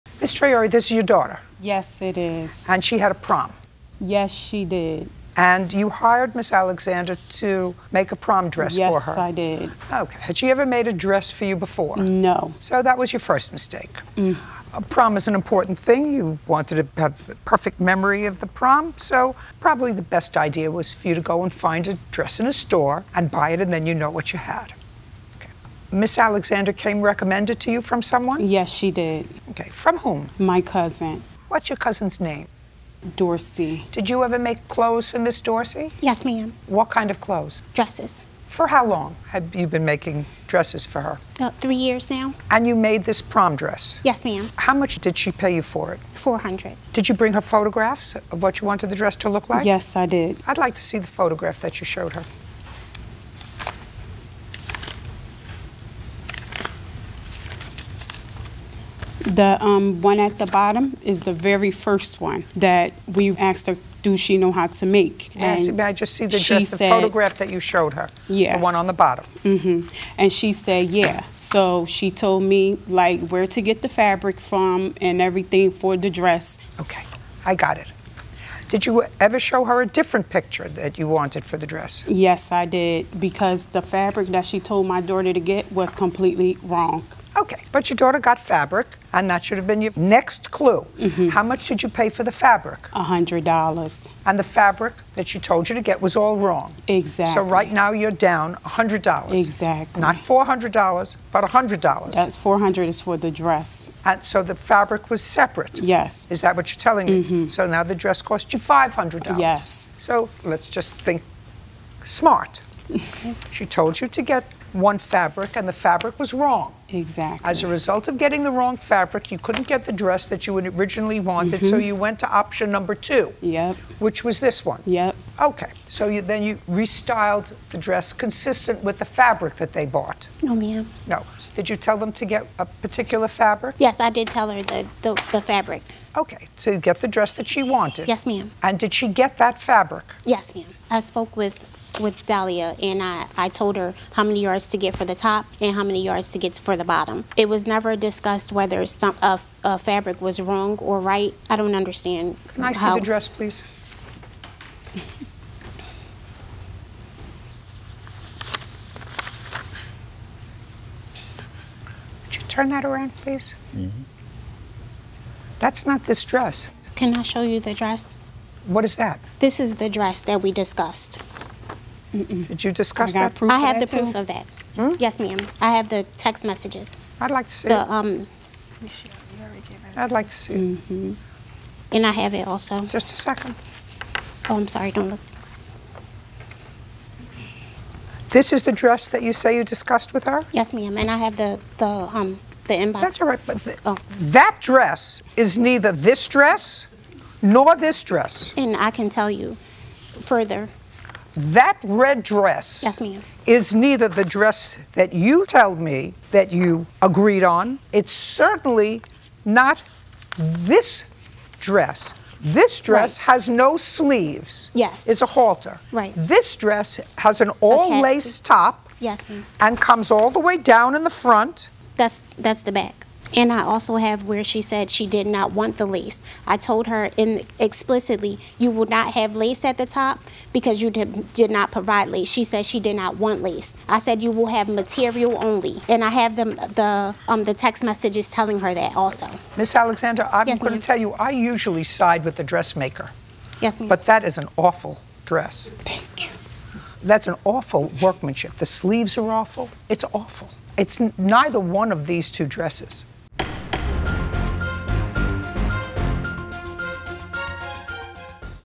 Did Seamstress' “Awful Dress” Ruin Prom? | Part 1
Florence hired seamstress Nichole to make a custom prom dress for her daughter Dalliyah – and regretted it!